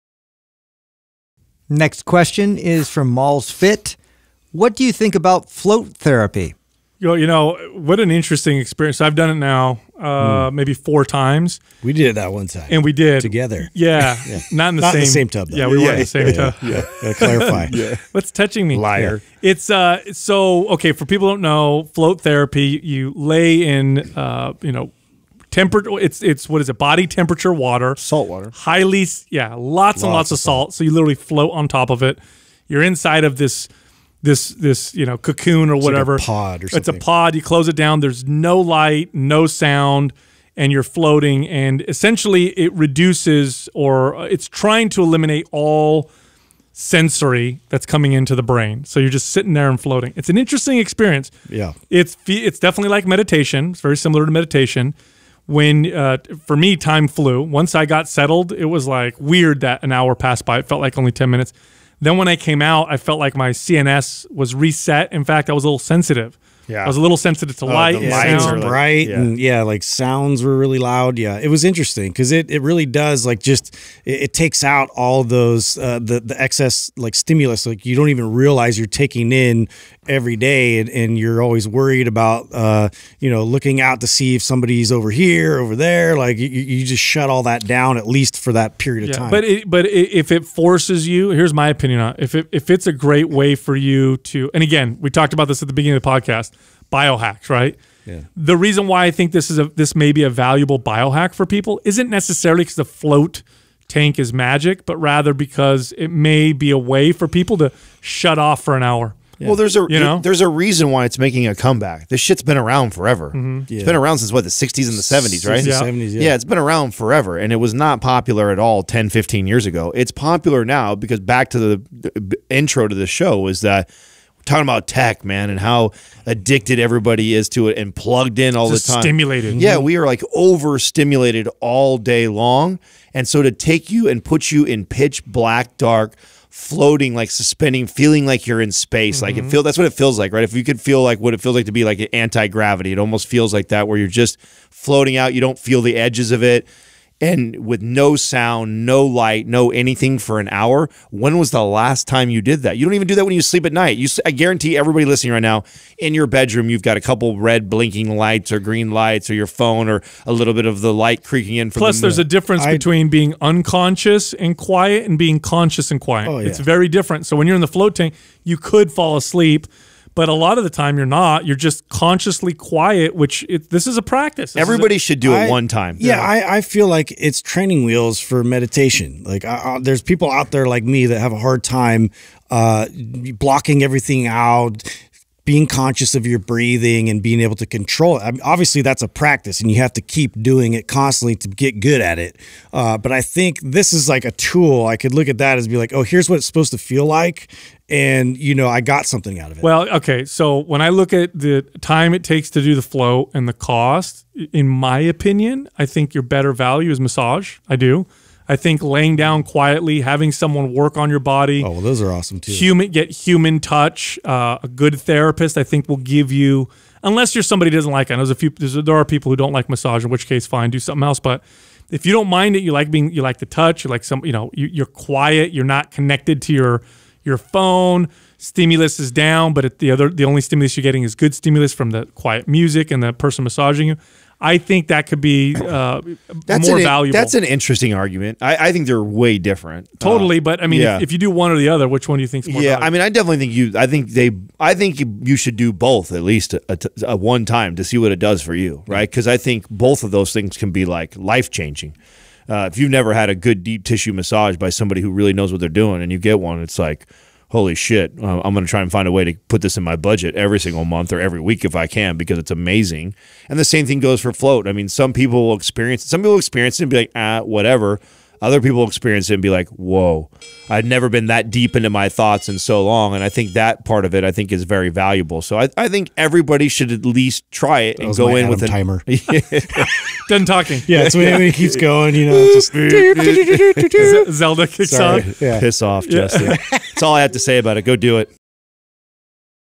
The Benefits of Float Therapy
In this QUAH Sal, Adam, & Justin answer the question “What do you think about float therapy?”